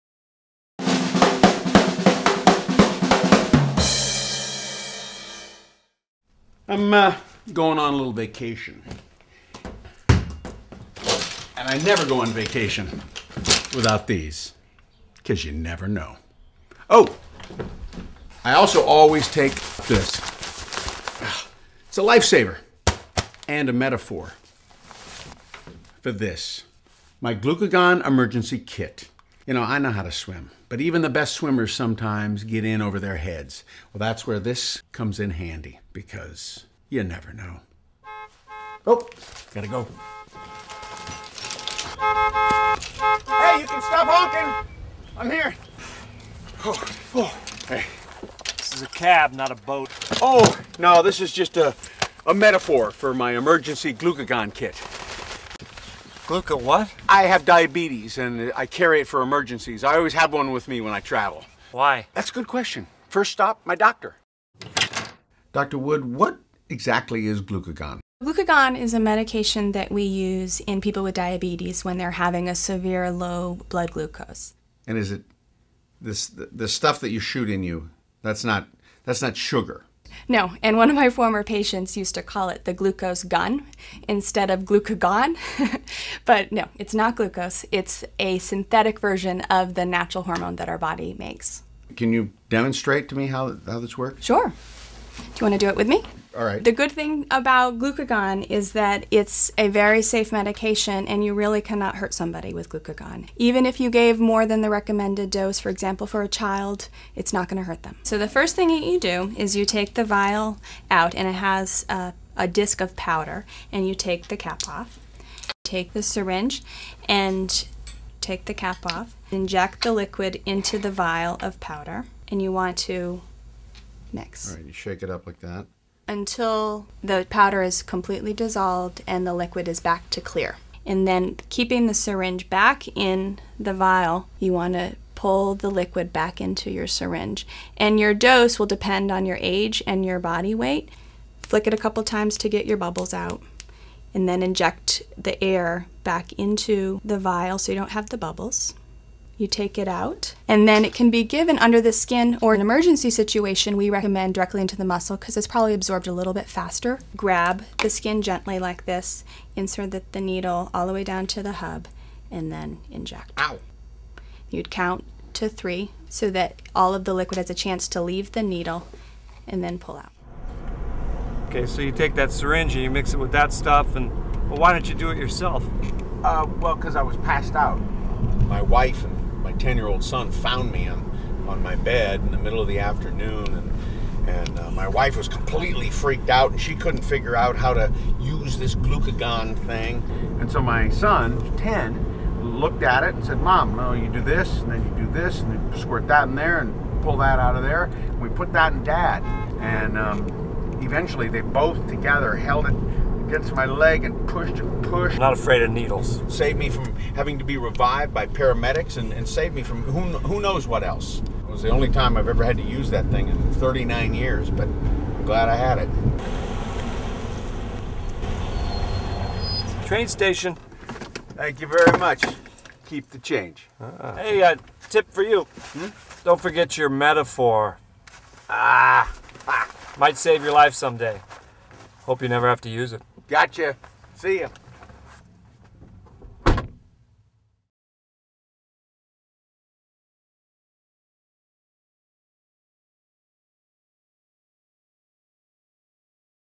Diabetes Emergency - Glucagon Kit Demonstration
Glucagon Demonstration for Diabetic Emergencies

VIDEO DESCRIPTION:
Jim Turner takes us through vacation packing procedures for people with diabetes.  The glucagon emergency kit is a must-have according to Jim and Doctor Jamie Woods.  Dr. Woods demonstrates the correct method of using the glucagon kit and shows that it is a real life-saver for those who have suffered the effects of severe low blood glucose.

Join the Pillbox Community